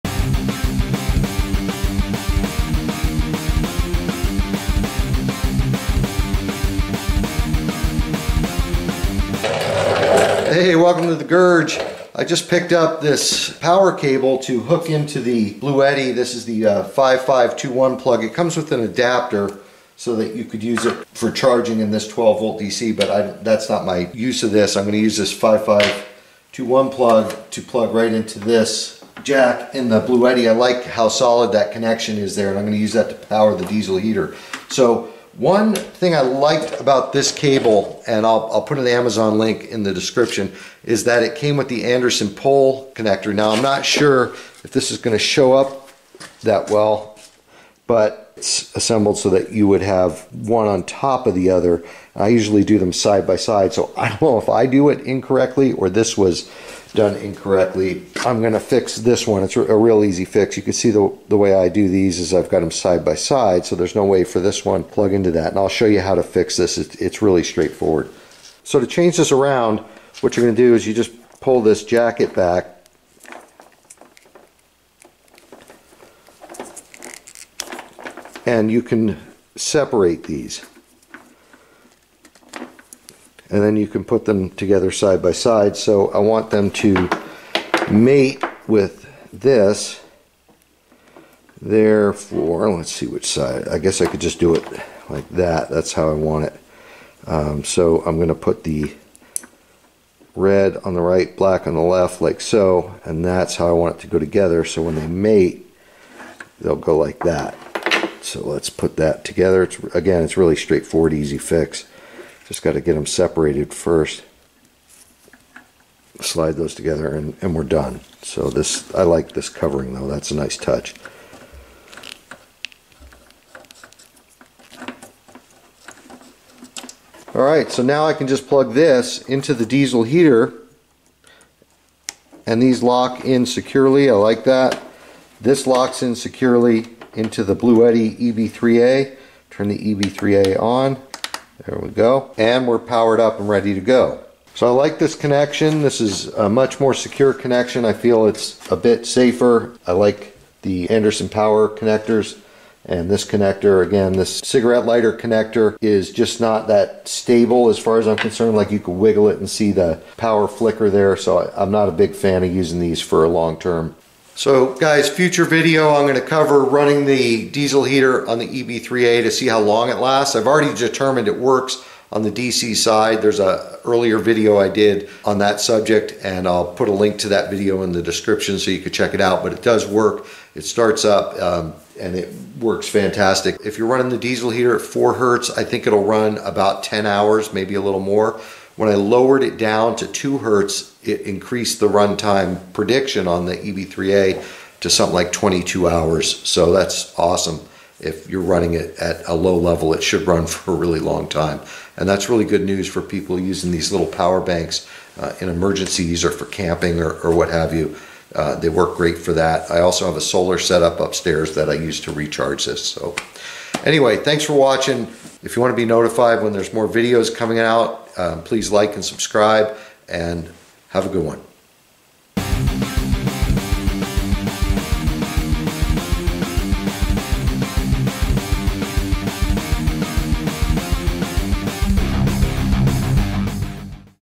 Diesel Heater DC Hookup to Power Bank - Bluetti EB3A
In this video I describe the hookup for using the DC side of a power bank (a Bluetti EB3A in this case) to power a diesel heater. I should have mentioned in the video that cable I'm using is 14g wire.

DC 5.5mm x 2.1mm Power Male Plug Cable

Vevor 5Kw Diesel Heater

Anderson Powerpole Connectors

Crimp tool for Powerpole (not needed, but nice to have)

Bluetti EB3A

Bluetti EB70 (higher capacity power bank)

Please note: What is presented here covers what I did to purchase, install and use this item. You should check your own local codes and consult a professional prior to using any item demonstrated in this video.